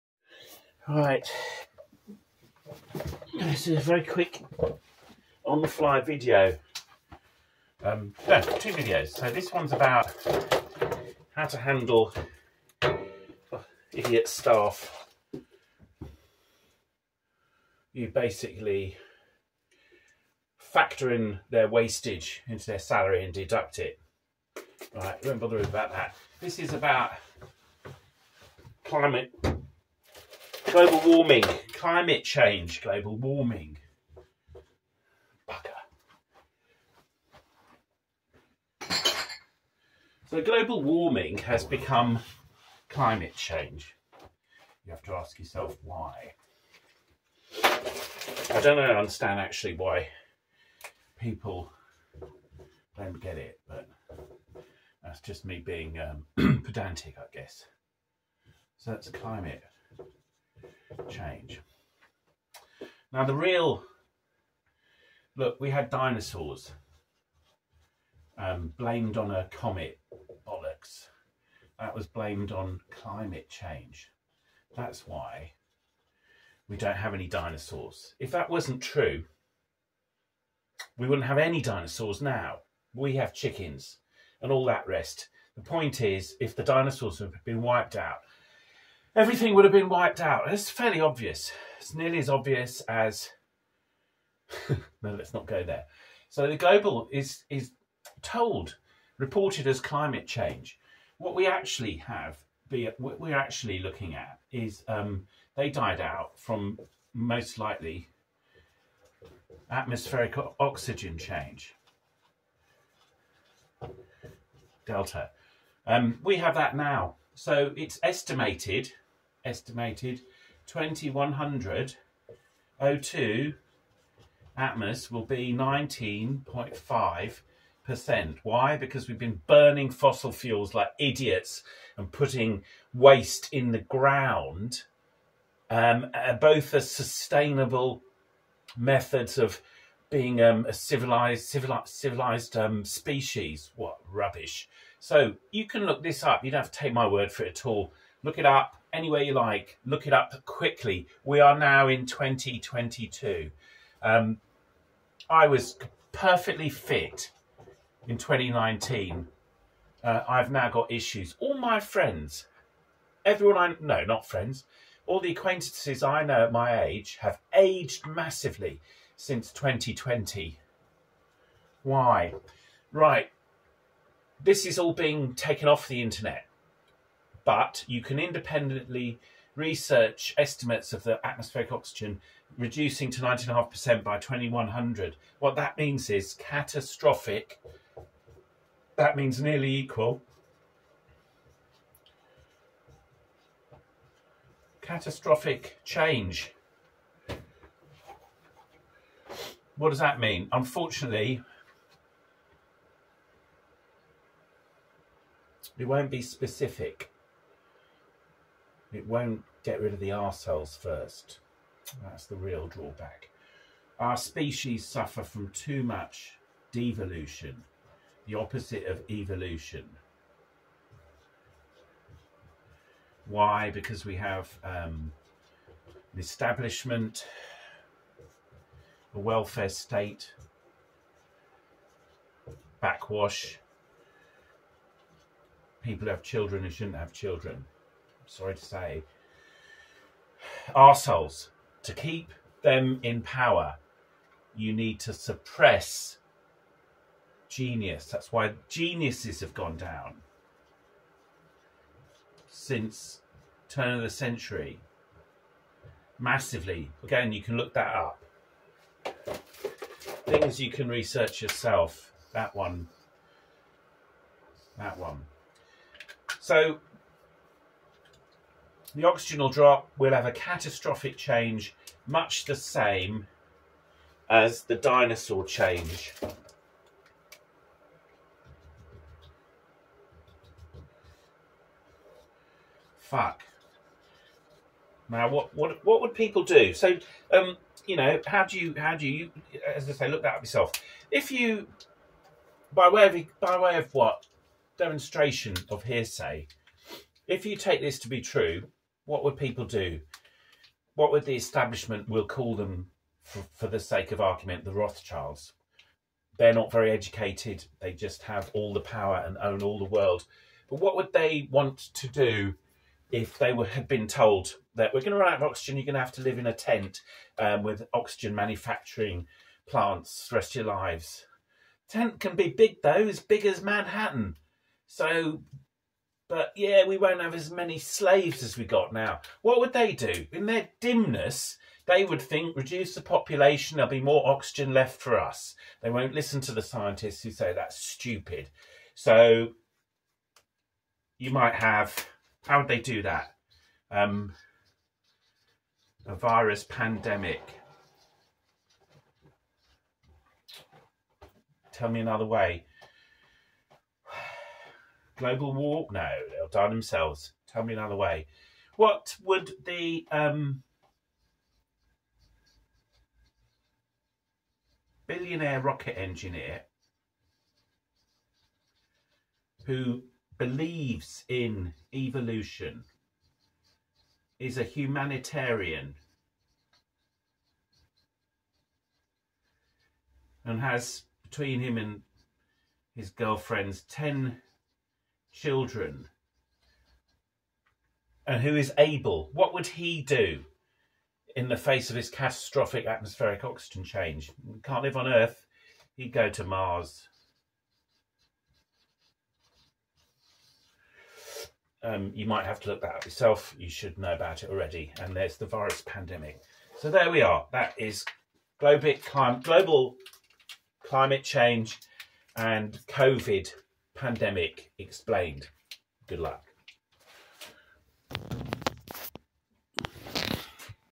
the reality & truth about global climate change and the 2020 covid pandemic(2)
this video briefly explains why dinosaurs were not knocked out by an atmospheric oxygen concentration with the next significant change estimated to occur at 2100.

we are told about global climate because the panic resulting from an atmospheric oxygen change would be unmanageable.

when faced with this catastrophe, what would the establishment do?

when faced with this catastrophe, what would the billionaire rocket engineer do?

you want to believe that not a single pair of dinosaurs survived an asteroid and that all life plants and animals after that milestones were of reduced size?

you want to believe that everyone is aging faster since 2020 because their exercise regime changed?

no need for panic: evolution will find a way. I'm likely descendent from an advanced ancient civilization.